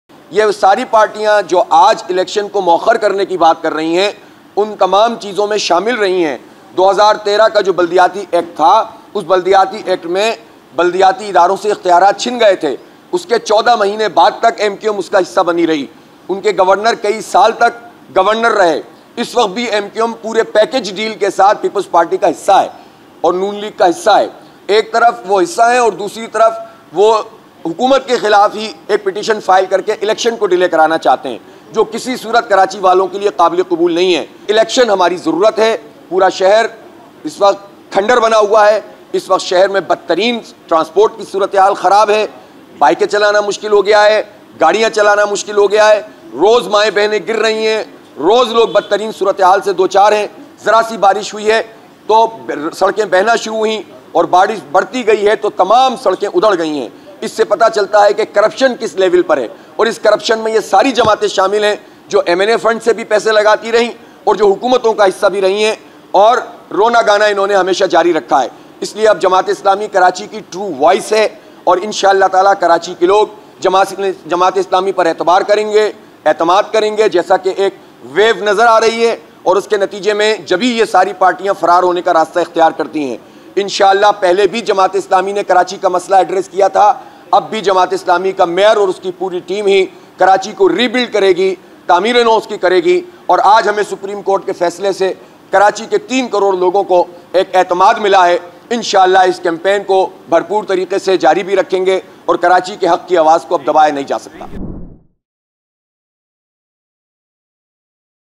جماعت اسلامی اور اس کی ٹیم ہی کراچی کو ری بلڈ کرے گی۔ حافظ نعیم الرحمن
الیکشن اس وقت کراچی کی ضرورت ہے، جماعت اسلامی کراچی کی آواز بن کر سامنے آئی ہے، جماعت اسلامی اور اس کی ٹیم ہی کراچی کو ری بلڈ کرے گی۔ حافظ نعیم الرحمن